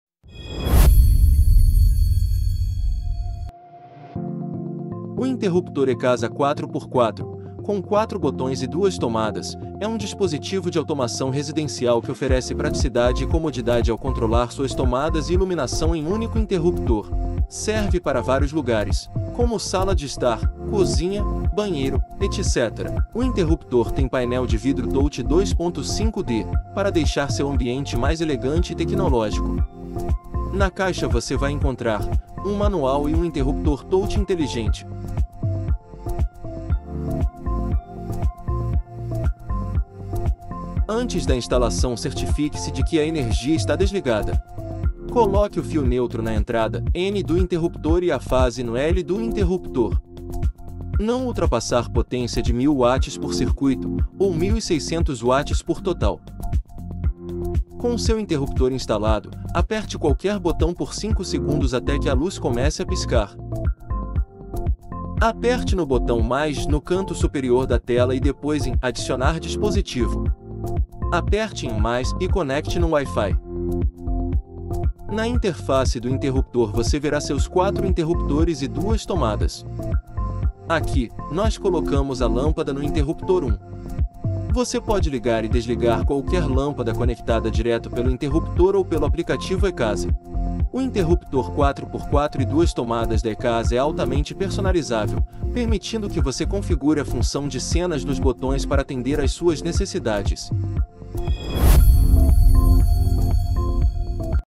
Interruptor INTELIGENTE 4x4 4 botões + 2 Tomadas - Esse aqui TEM MUITAS funcionalidades!!
Esperamos que você tenha gostado desse review do interruptor inteligente 4x4 T-3075 , não se esqueça de colocar aqui nos comentários o review do produto que você quer ver por aqui!!

Para saber mais sobre o controle ou sobre os nossos produtos para casa inteligente acesse o nosso